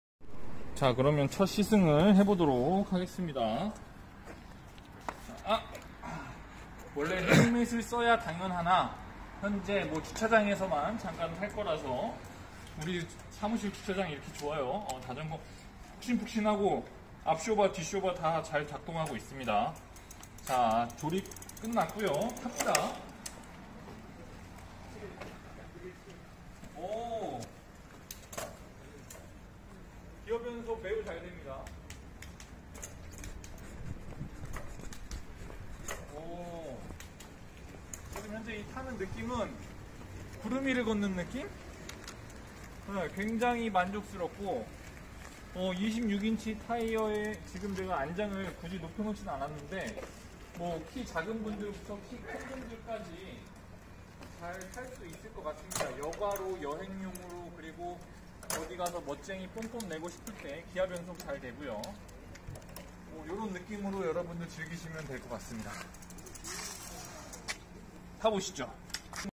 10만원대 자전거 심천잡스 광폭 타이어 자전거 팻바이크 구매대행
코로나 시대 우리는 자전거로 운동과 이동과 즐거움을 느껴야한다고 생각합니다.